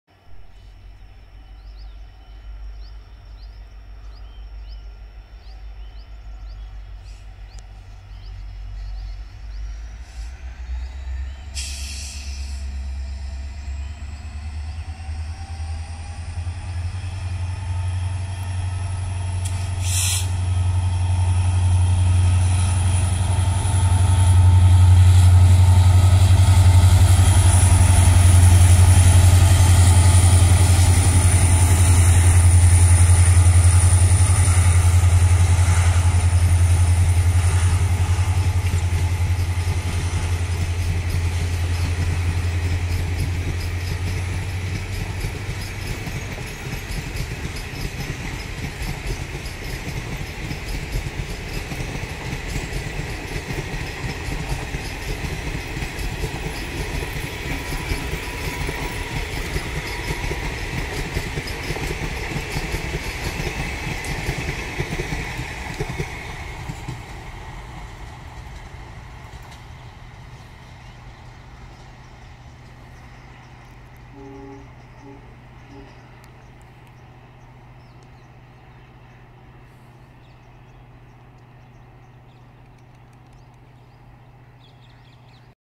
02701 Mumbai - Hyderabad Hussain Sagar Super Fast Express Departure Gulbarga With Gooty WDP-4D & AJJ
AJJ WAP-4